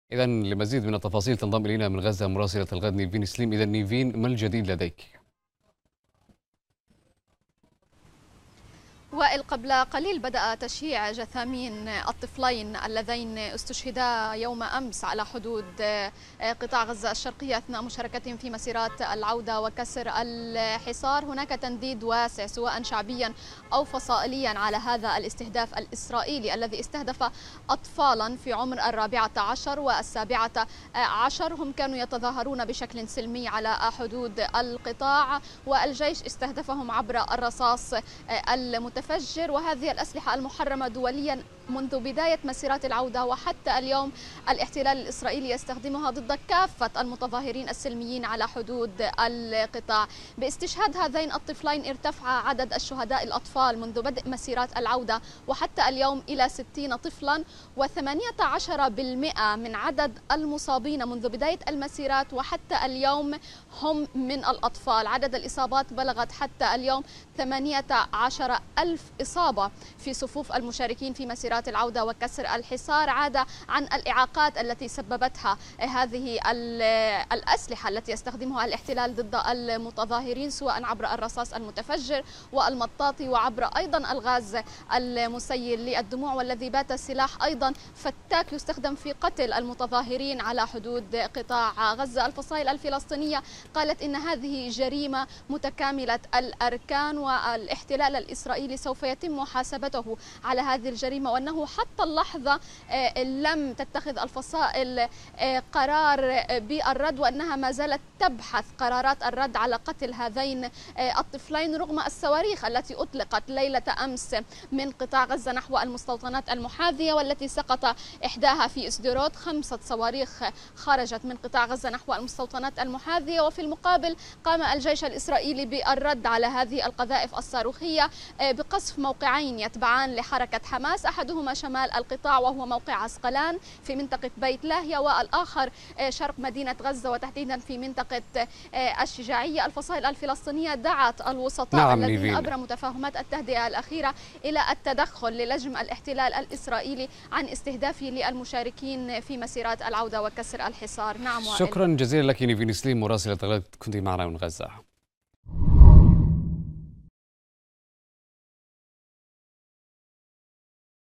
غزة تشيع جثامين الطفلين الشهيدين في الجمعة 73 من مسيرات العودة.. إليك التفاصيل
أفاد مراسل الغد باستهداف الاحتلال موقعين  للفصائل الفلسطينية في  مدينة غزة

وأضاف مراسلنا أن صافرات الإنذار
دوت في منطقة غلاف غزة‬ بجانب نشوب حريق قرب مستوطنة اسدروت جراء سقوط صاروخ في منطقة مفتوحة.

 وزعم  جيش الاحتلال أنه رصد إطلاق 5 قذائف صاروخية من القطاع تجاه المستوطنات.
المزيد من التفاصيل يتحدث مراسل قناة الغد من غزة، نيفين اسليم، عبر النشرة الإخبارية.